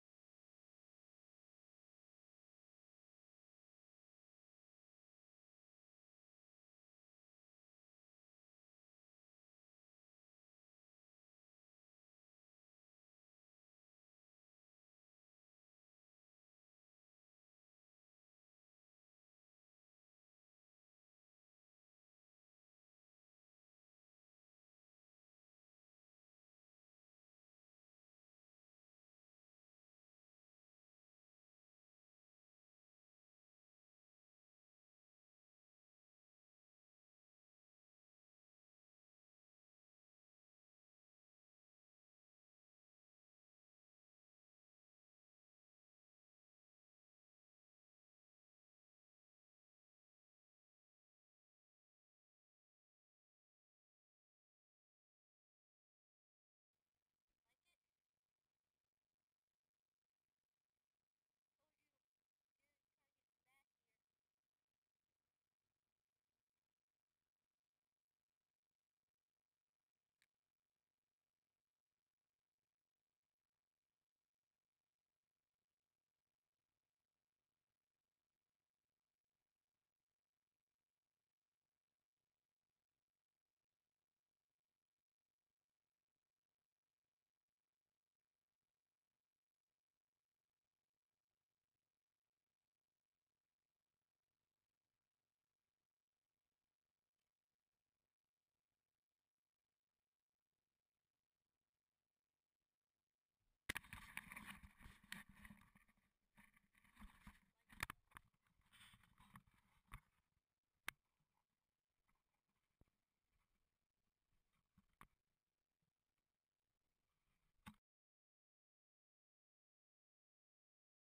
Bass Fishing
Had a good time catching the bass. Sorry that i didnt post in a while.